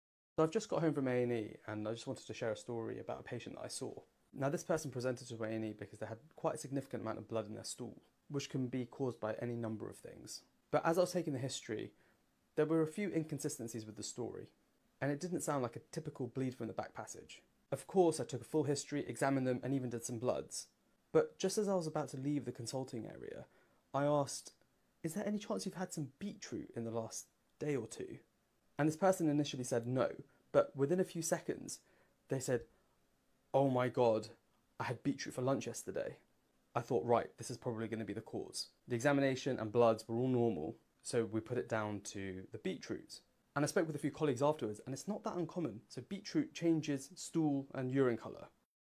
A day in the life of a doctor | blood in poo
Have you ever had blood in your poo? doctorexplains patientstories bloodinstool beetroot poo PS. Always speak to a medical practitioner if you’re worried about blood in your stool.